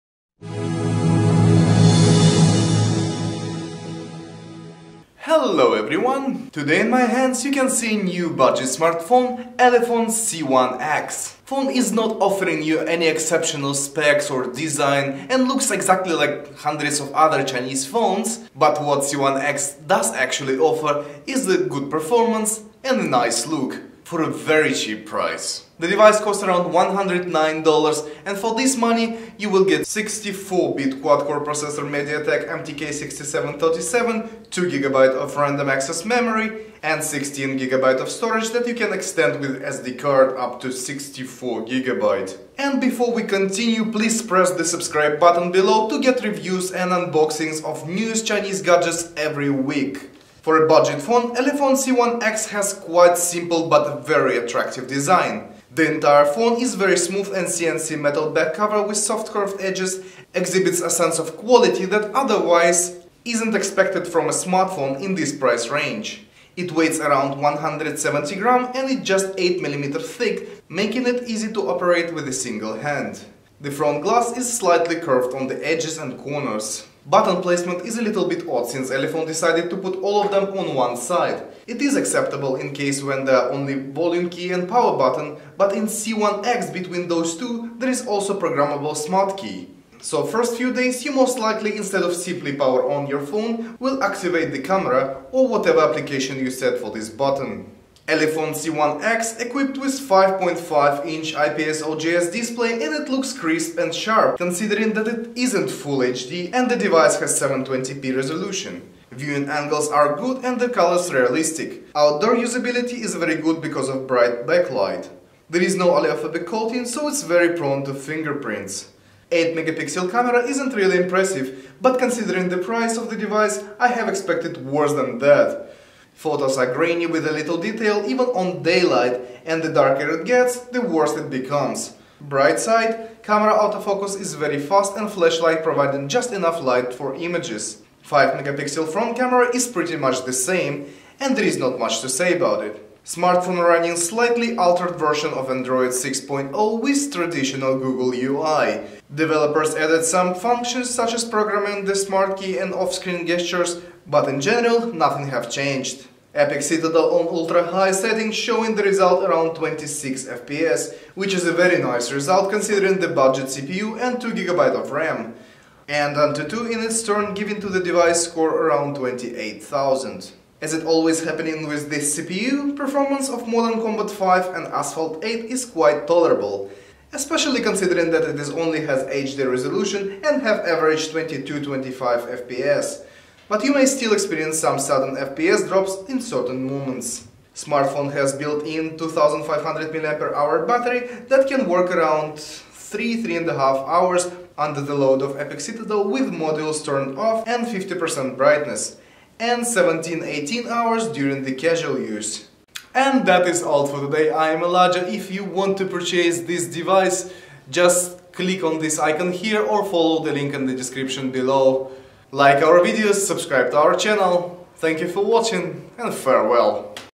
"Elephone C1X" Review : Good looking budget smartphone
Elephone C1X Highlights

Main Features:
Display: 5.5 inch 2.5D Arc Screen 1280 x 720 pixels HD screen
CPU: MTK6737 Quad Core 1.3GHz
System: Android 6.0
Storage: 2GB RAM + 16GB ROM
Cameras: 5.0MP front camera + rear camera 8.0MP ( SW 13.0MP ) with flash light and AF
Bluetooth: BT4.0
Features: Gravity Sensor, Proximity Sensor, Light Sensor, Fingerprint Scanner
SIM Card: dual SIM dual standby, nano SIM + micro SIM

Network:
2G: GSM 850/900/1800/1900MHz
3G: WCDMA 900/2100MHz
4G: FDD-LTE 800/1800/2100/2600MHz
TD-LTE B40

-Click Thumb Up if you liked our video!